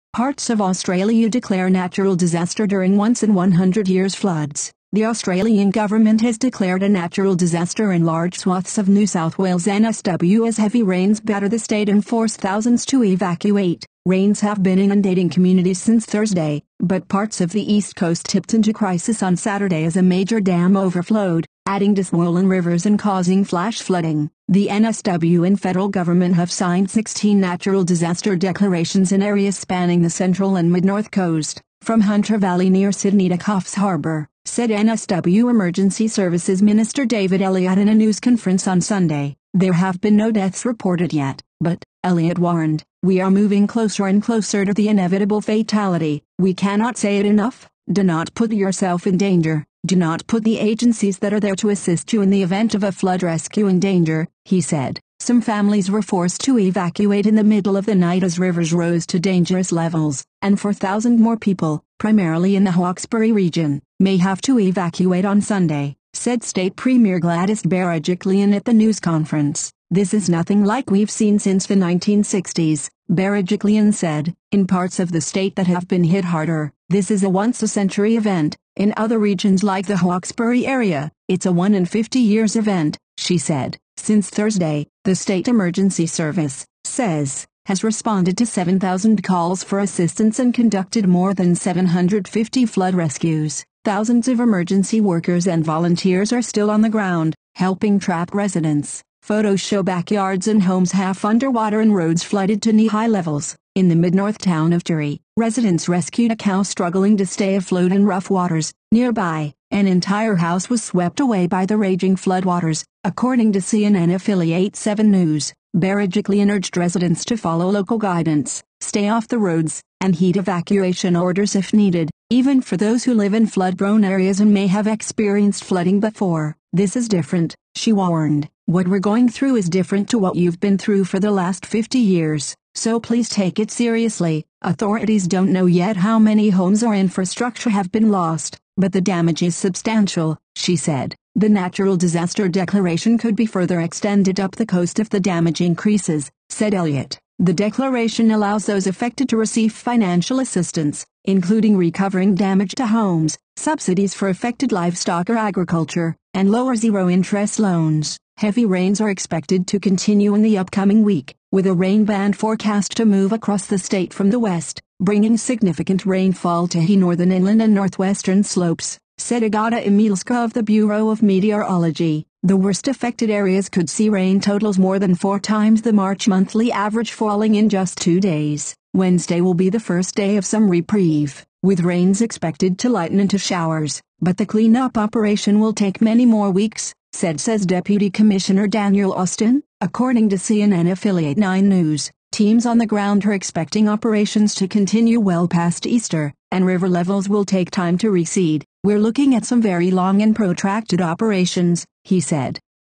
Parts of Australia declare natural disaster during 'once in 100 years' floods
The Australian government has declared a natural disaster in large swaths of New South Wales (NSW) as heavy rains batter the state and force thousands to evacuate.
Rains have been inundating communities since Thursday, but parts of the east coast tipped into crisis on Saturday as a major dam overflowed, adding to swollen rivers and causing flash flooding.
The NSW and federal government have signed 16 natural disaster declarations in areas spanning the central and mid-north coast, from Hunter Valley near Sydney to Coff's Harbour, said NSW Emergency Services Minister David Elliott in a news conference on Sunday.
There have been no deaths reported yet -- but, Elliott warned, "we are moving closer and closer to the inevitable fatality."
"We cannot say it enough: do not put yourself in danger, do not put the agencies that are there to assist you in the event of a flood rescue in danger," he said.
Some families were forced to evacuate in the middle of the night as rivers rose to dangerous levels, and 4,000 more people -- primarily in the Hawkesbury region -- may have to evacuate on Sunday, said state Premier Gladys Berejiklian at the news conference.
"This is nothing like we've seen since the 1960s," Berejiklian said. In parts of the state that have been hit harder, this is a once-a-century event; in other regions like the Hawkesbury area, it's a "one-in-50-years" event, she said.
Since Thursday, the State Emergency Service (SES) has responded to 7,000 calls for assistance and conducted more than 750 flood rescues. Thousands of emergency workers and volunteers are still on the ground, helping trapped residents.
Photos show backyards and homes half underwater and roads flooded to knee-high levels. In the mid-north town of Taree, residents rescued a cow struggling to stay afloat in rough waters; nearby, an entire house was swept away by the raging floodwaters, according to CNN affiliate Seven News.
Berejiklian urged residents to follow local guidance, stay off the roads, and heed evacuation orders if needed -- even for those who live in flood-prone areas and may have experienced flooding before. "This is different," she warned. "What we're going through is different to what you've been through for the last 50 years. So please take it seriously."
Authorities don't know yet how many homes or infrastructure have been lost, but "the damage is substantial," she said.
The natural disaster declaration could be further extended up the coast if the damage increases, said Elliott. The declaration allows those affected to receive financial assistance, including recovering damage to homes, subsidies for affected livestock or agriculture, and low- or zero-interest loans.
Heavy rains are expected to continue in the upcoming week, with a rain band forecast to move across the state from the west, bringing significant rainfall to he northern inland and northwestern slopes, said Agata Imielska of the Bureau of Meteorology. The worst-affected areas could see rain totals more than four times the March monthly average falling in just two days.
Wednesday will be the first day of some reprieve, with rains expected to lighten into showers.
Teams on the ground are expecting operations to continue "well past Easter," and river levels will take time to recede. "We're looking at some very long and protracted operations," he said.